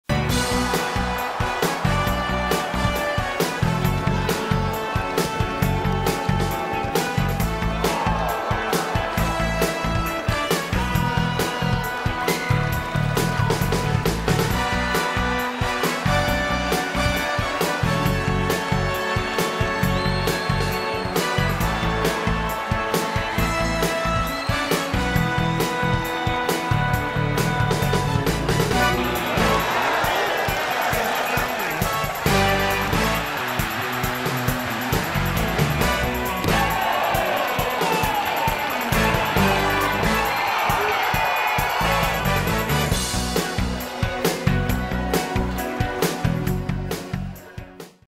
Приёмы дзюдо на детских соревнованиях в Болгарии. Приём 2
Фрагменты приёмов дзюдо на чемпионате Болгарии до 15 лет в городе Димитровград 2024